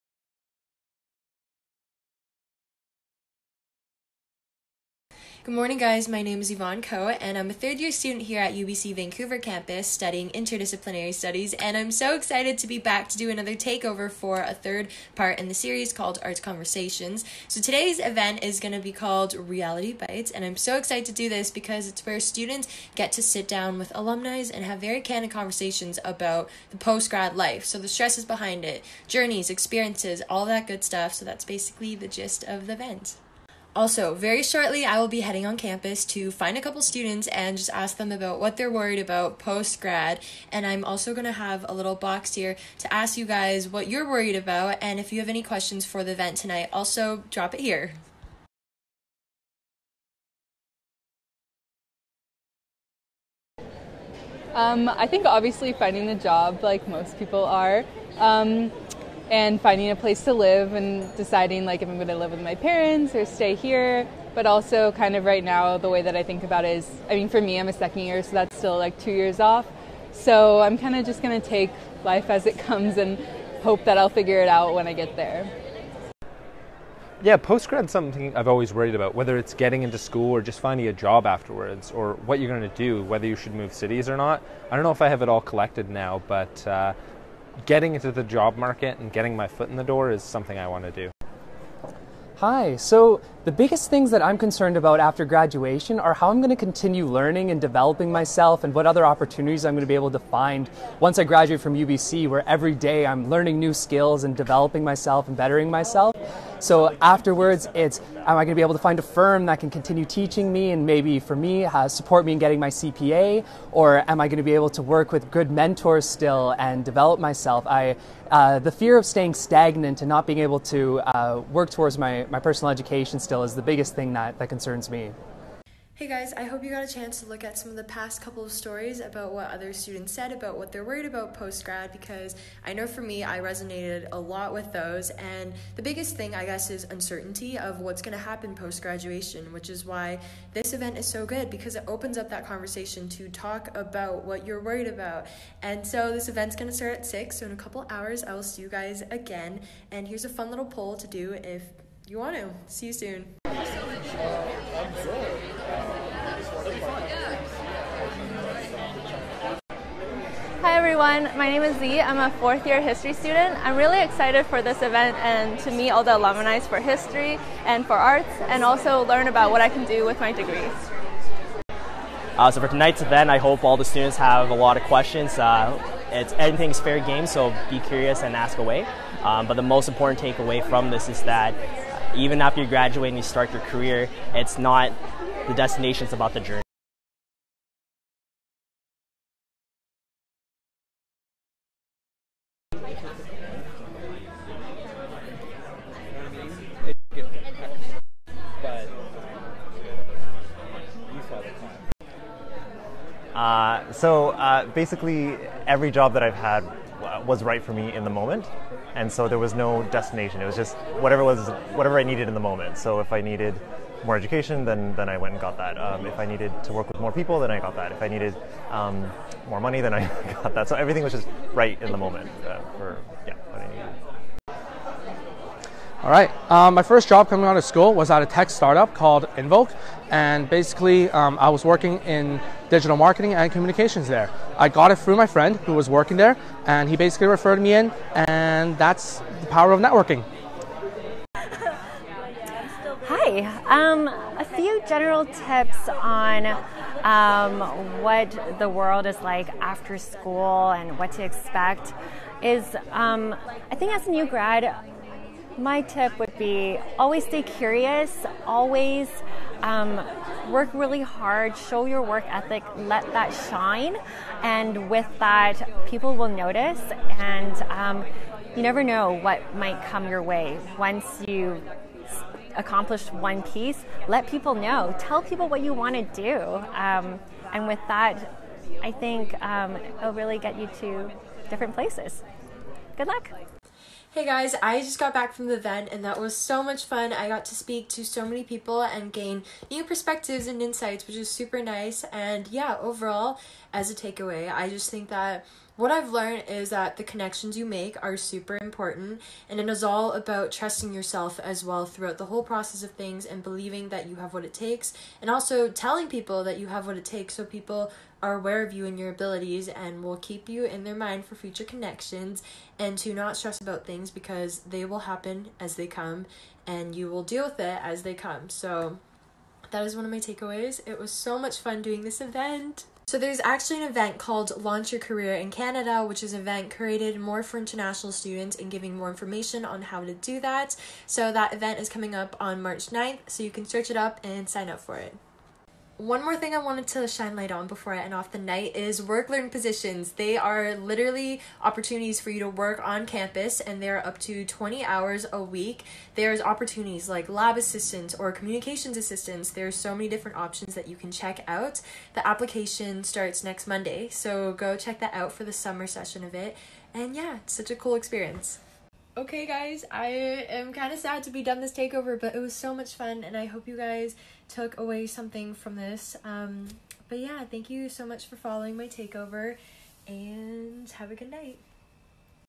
Student Takeover with Yvonne: Reality Bites
Follow along as third-year Arts student Yvonne Ko takes over our Instagram STORIES and brings us to another alumni-student networking event: Reality Bites. ⁠
Part of Arts Career Conversations series, students will get to have candid conversations over dinner with alumni and learn about the surprising ways that failure can lead to success. The event was on Tuesday, March 3, in the Robert H.Lee Alumni Centre. ⁠
About Yvonne: Hey everyone! I'm Yvonne and I'm a third year student at UBC studying Interdisciplinary Studies with a focus in creative writing and film studies! Besides applying my passions of creating within my studies, I also share my life as a student on my YouTube channel (Itsyvn). I'm so excited to be doing a takeover for this event as it'll shine light on life after graduation (which is something I'm sure we're all thinking about!) See you then!⁠

To Learn more about our event "Arts Career Conversations: Reality Bites," visit this